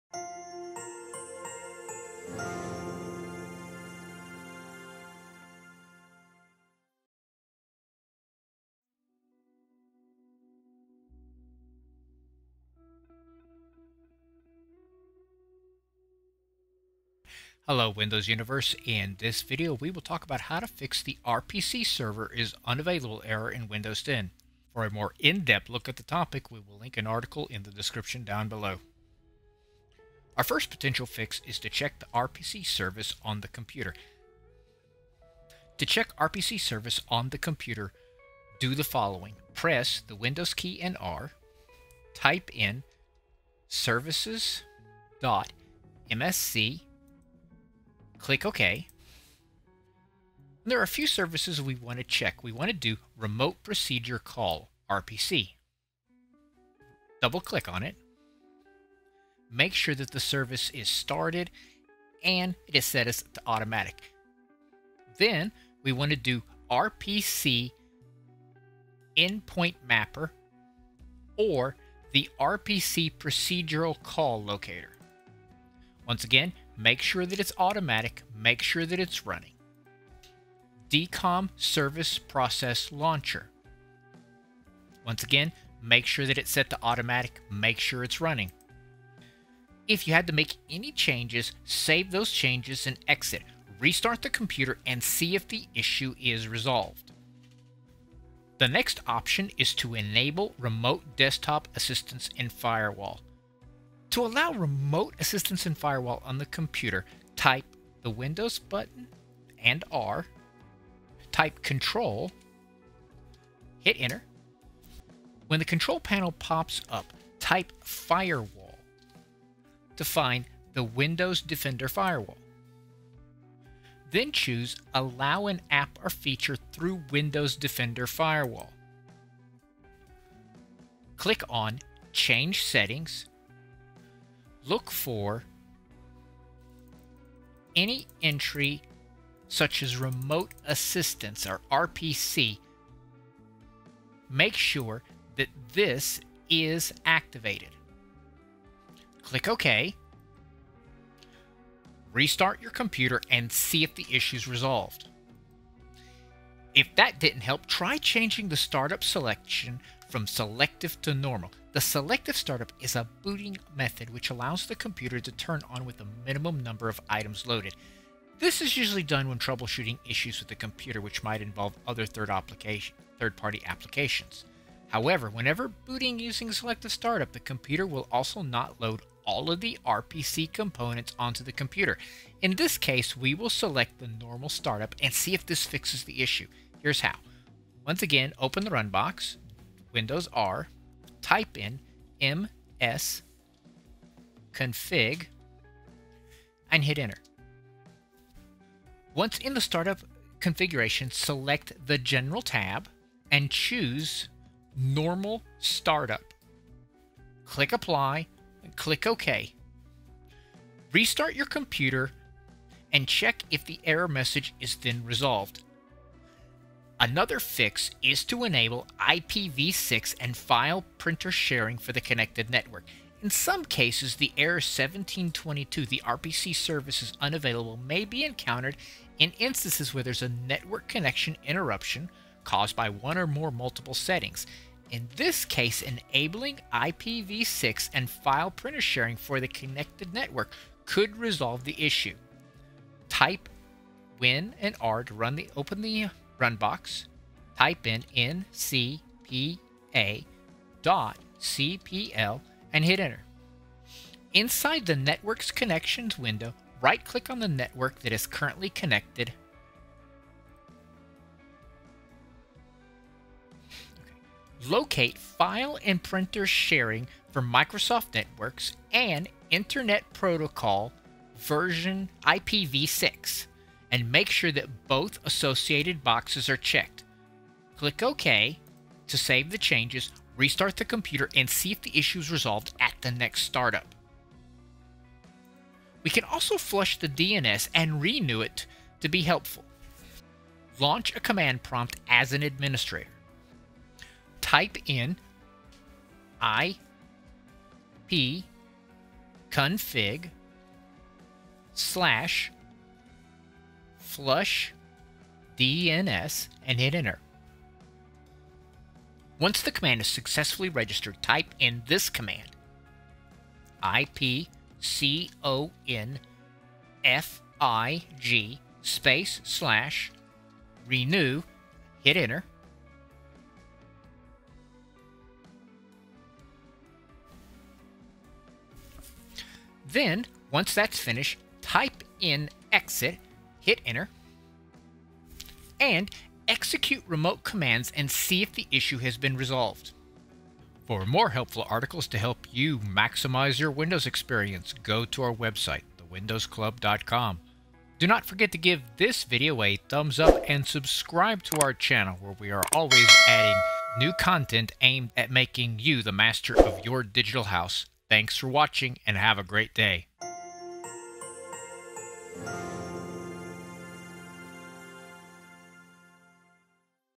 Fix The RPC server is unavailable error in Windows 11
Remote Procedure Call (RPC) is a technology that has existed since the emergence of computers and utilizes the interprocess communication technique. Its primary purpose is to enable a client and a server to communicate over the network. After completing an upgrade to a newer version of Windows 11/10, some users might notice that executing any remote command fails with the error message The RPC server is unavailable – prior to the upgrade, these remote commands execute successfully. In this post, we will identify the potential causes and then provide the possible solutions that you can try to help remediate this issue.

If you face this error, you can try the recommended solutions below.

1] Check RPC service on your computer

2] Enable Remote Desktop/Assistance in Firewall

3] Change Startup Selection from Selective startup to Normal startup

4] Enable IPV6 and File & Printer Sharing for the connected network

5] Flush DNS & Renew

6] Force RPC services to start using Registry Editor